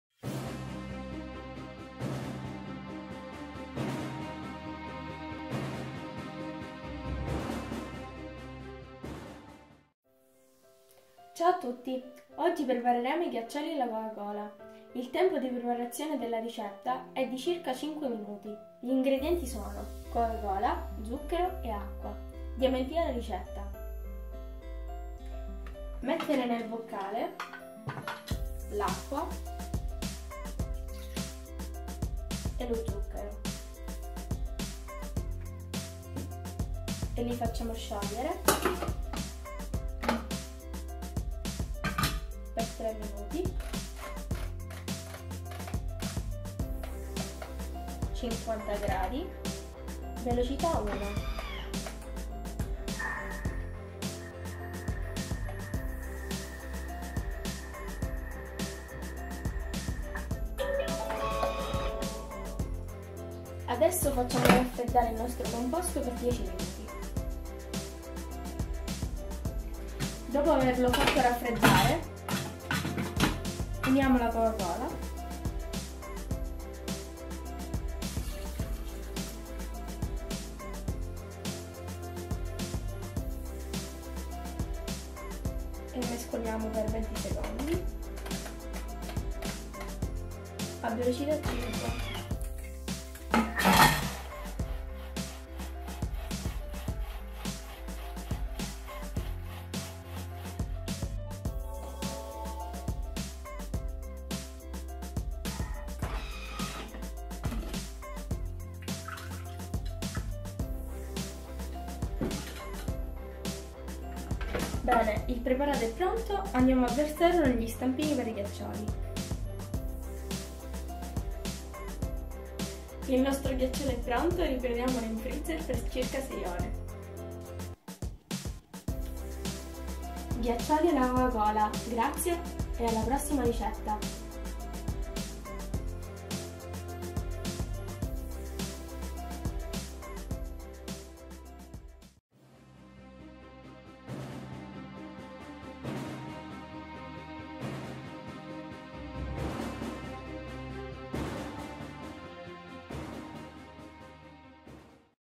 Ghiaccioli alla coca cola per bimby TM6 TM5 TM31 TM21
Ingredienti:
300 g di coca cola
100 g di acqua
20 g di zucchero